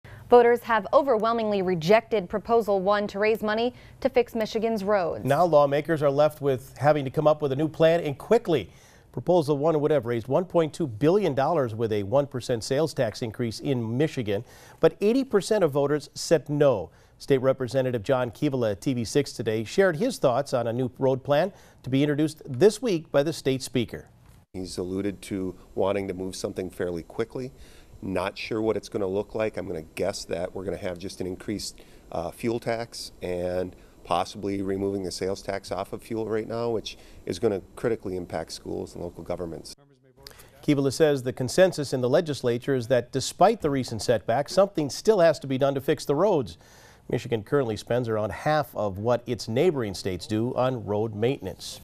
Lawmakers working on new plan for Michigan roads
Voters have overwhemingly rejected Proposal 1 to raise money to fix Michigan's roads